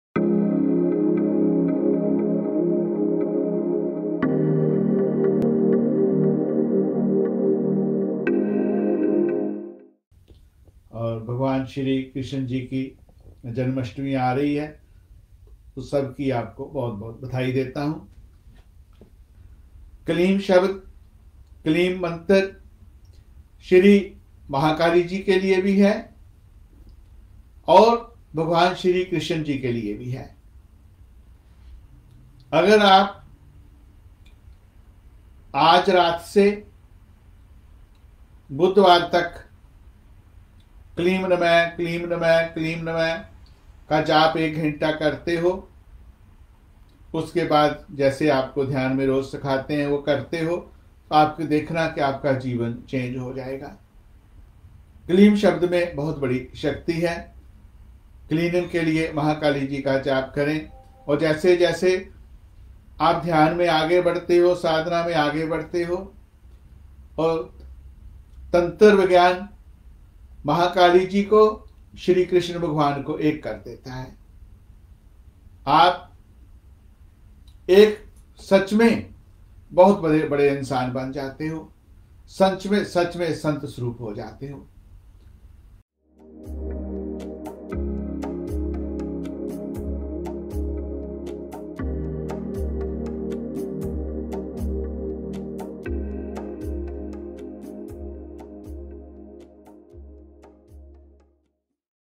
Powerful Janmashtami Sadhana to Change Your Life
Lord Krishna’s Birthday (Janmashtami) is tomorrow. In this video, Swami Ji shares with us a powerful Mantra Sadhana that merges the powers of Maha Kali and Lord Krishna.

This Mantra combined with your regular Sadhana will change your life in wondrous ways. Try this and share your experience with us.